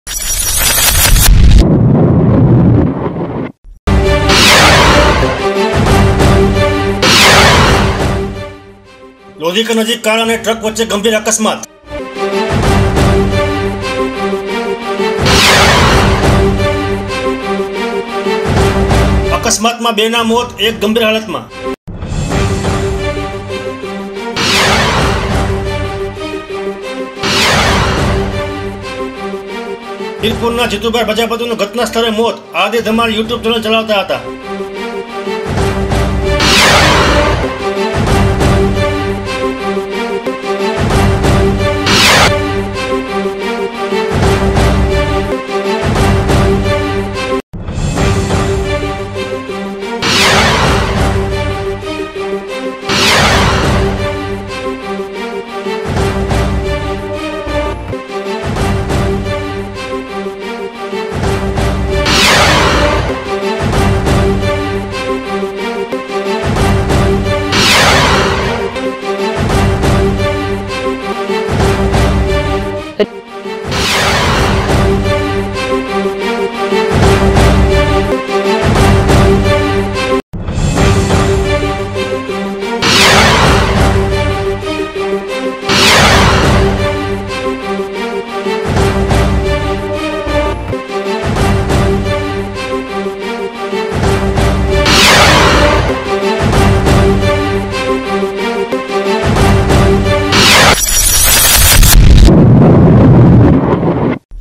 લોધીકા નજીક કાર અને ટ્રક વચ્ચે અકસ્માત વીરપુર ના જીતુભાઇ પ્રજાપતિ નું ઘટના સ્થરે મૌત
Editor : Ramesh Dhakan
Sub Editor : Satish Vadukar
Managing Editor : RG Sarvaiya